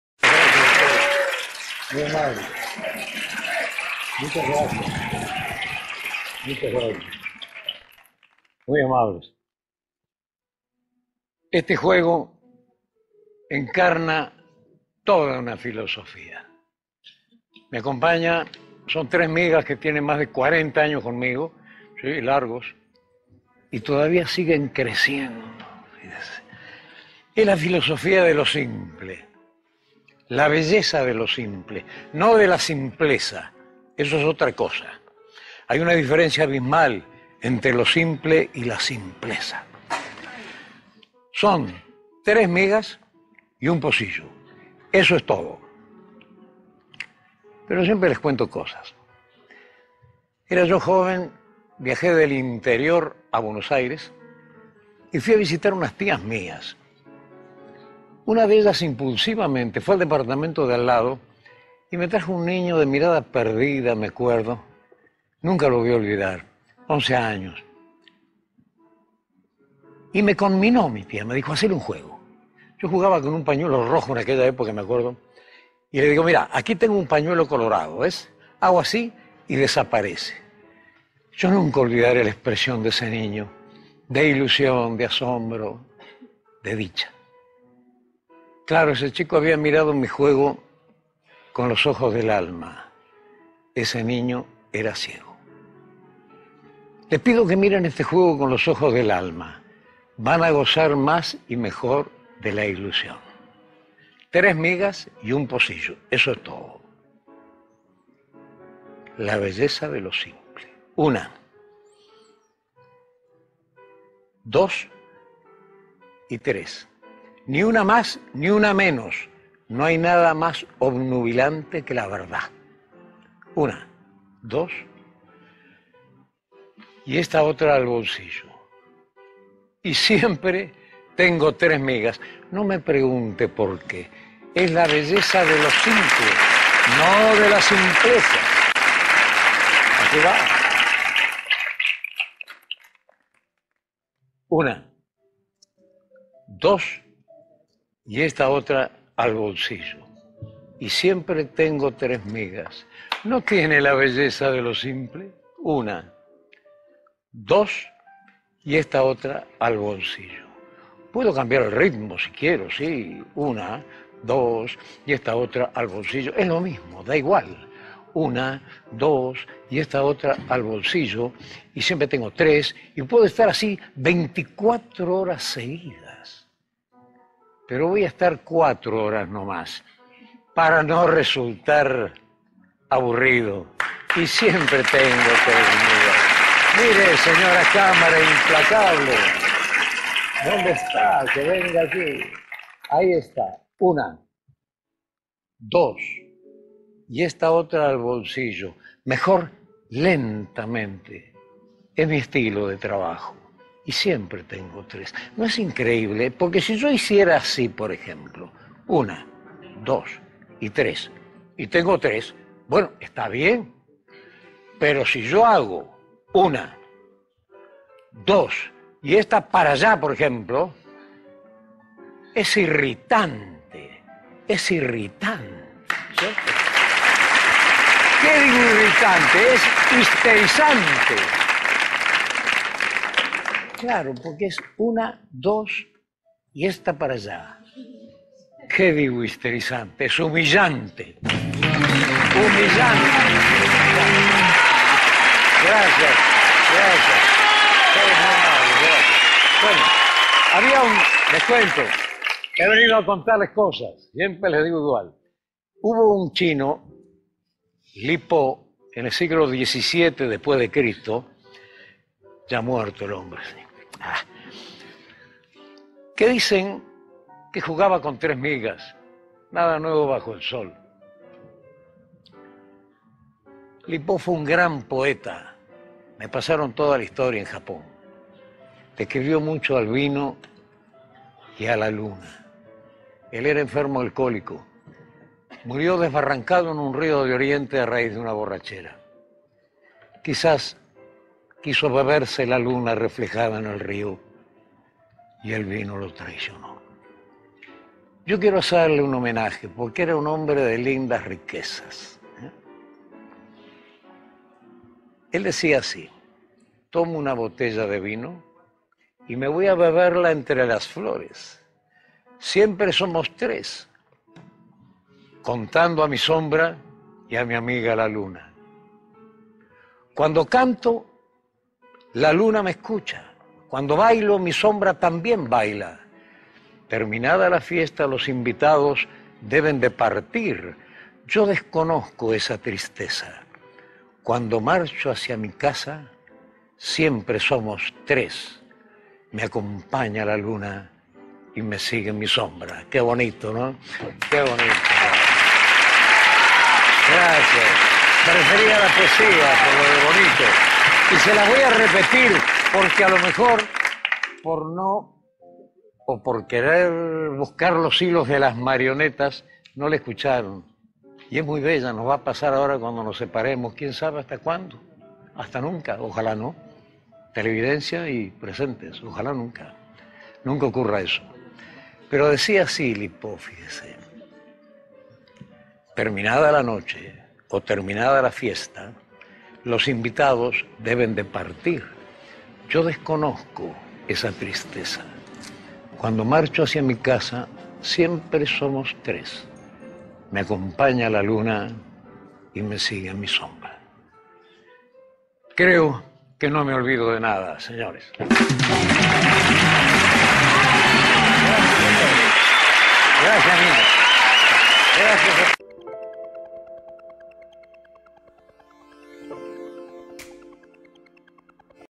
Rene Lavand - La belleza de lo simple
El maravilloso mago con una mano Rene Lavand nos trae un juego la belleza de lo simple